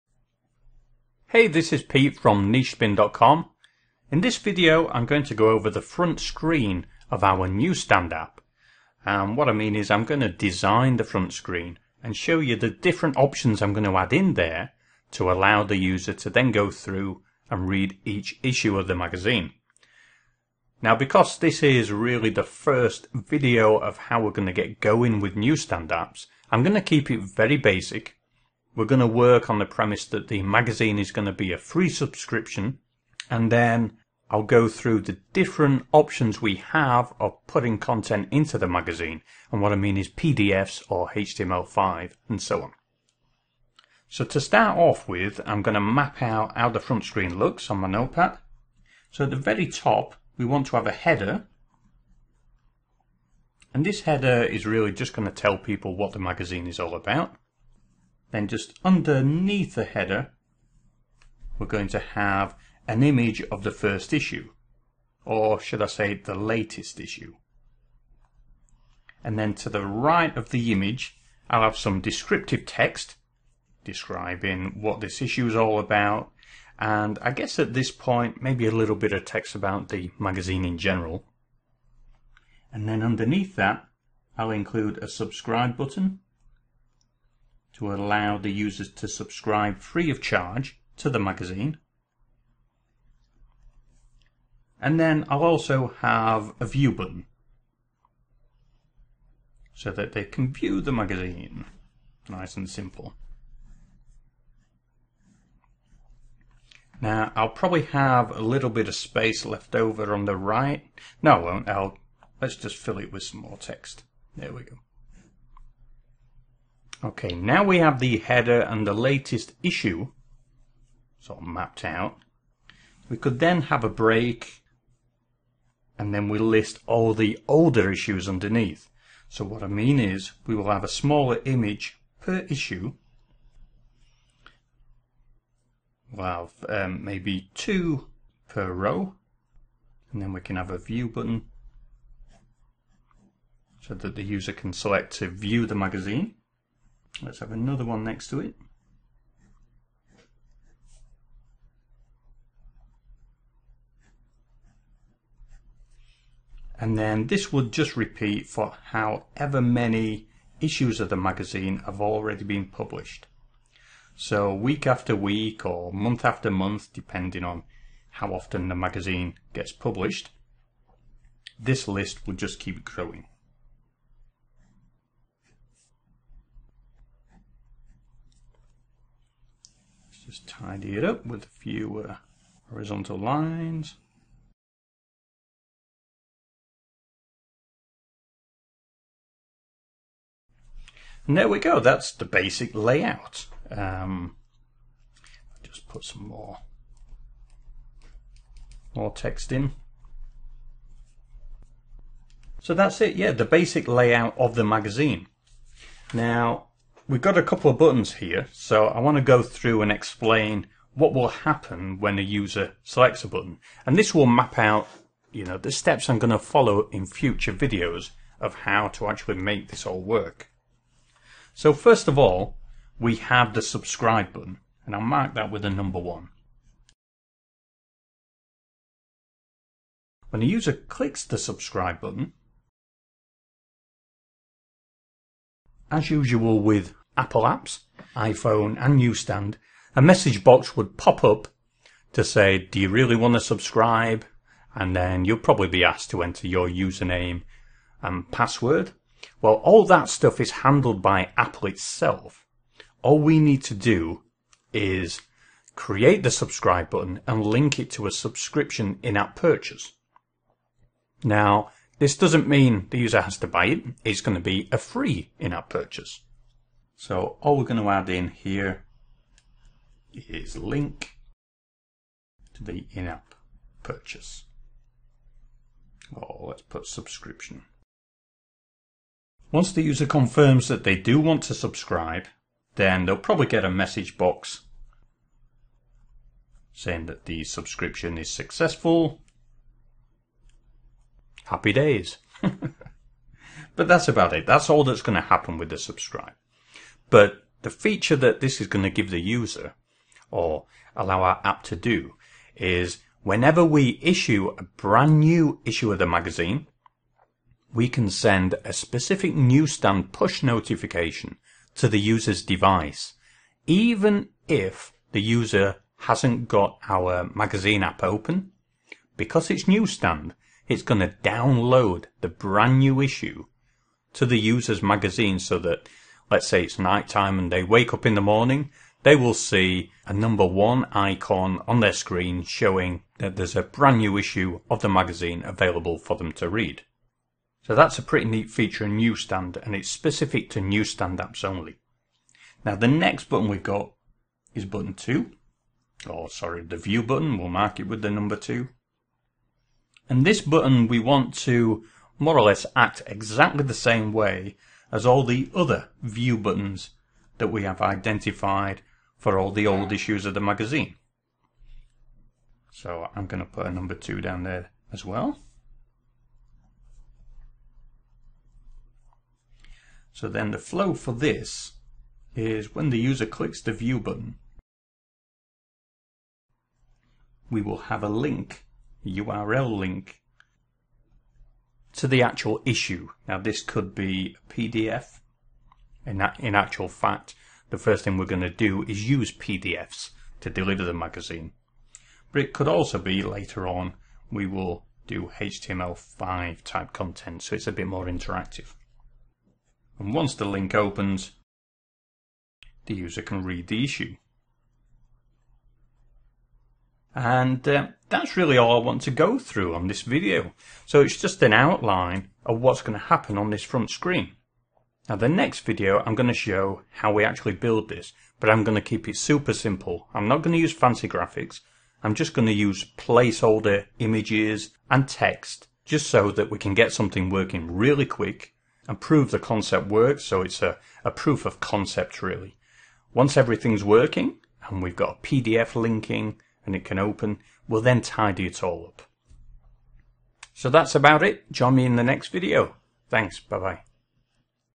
NewsStand App - Front Screen Design Layout
Design And Build a Apple Newsstand App - Tutorial showing the basic front screen design layout for the magazine newsstand app I am building and documenting

This video shows the subscription and view button designs for the in-app purchases.